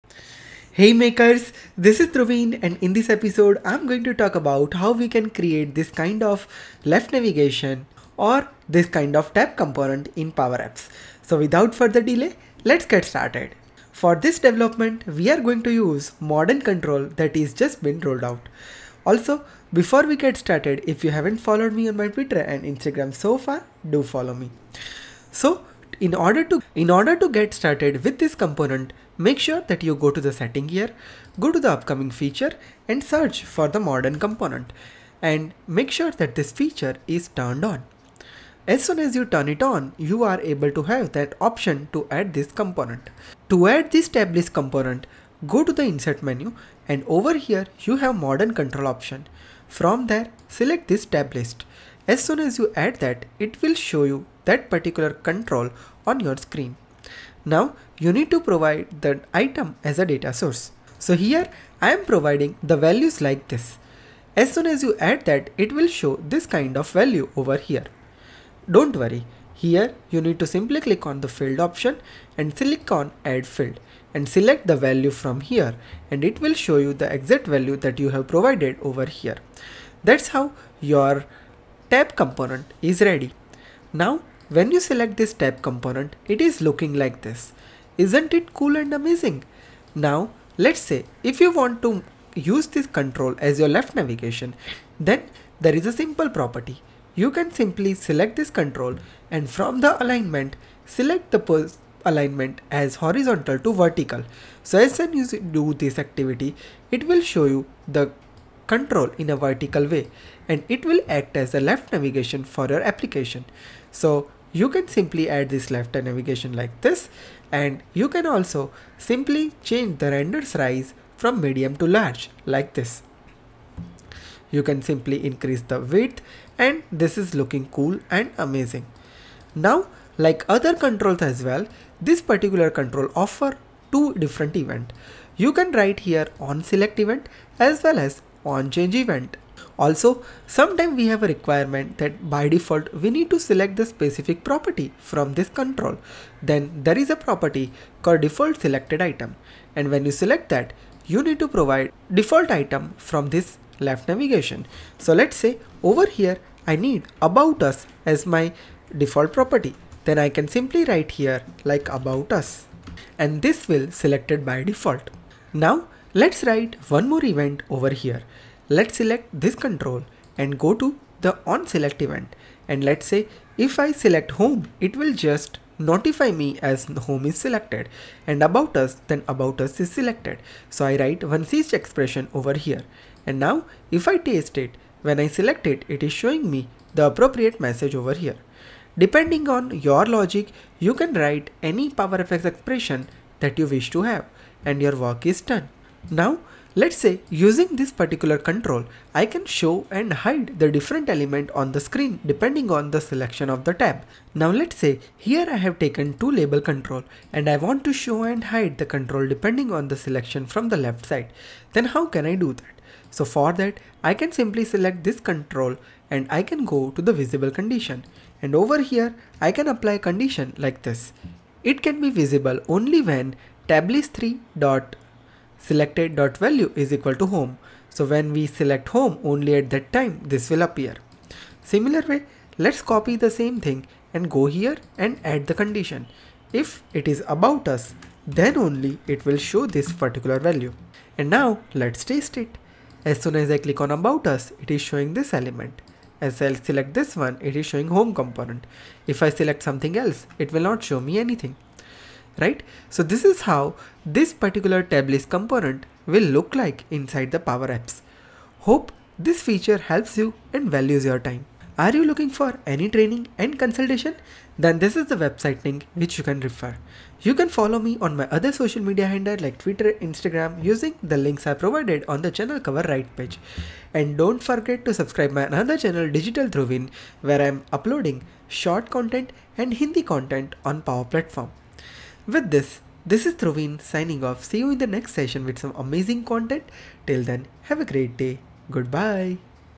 Modern Tablist Control for Tabs & Left Navigation in Power Apps
This video will teach us about one of the latest Modern controls from Power Apps Canvas App. We will talk about Tablist control in Canvas App. Tablist control in Power Apps Canvas app is used to create some types of Left navigation to Tab control for your screen! The Modern Control in Power Apps is in Preview at this moment! Here, we will talk about the different properties of Tablist control and learn how we can use them in real projects. So, let's get started!

Chapters:
00:00 Start
00:29 Enable Modern Controls
00:50 Add Tablets Component in Power Apps Screen
01:10 Configure Field in Tablets
02:00 Different Configuration Property
02:15 Events for Tab control
02:30 Default Selected Item for Tablets control
02:46 OnSelect Event
03:17 Show/Hide Values for Tablets control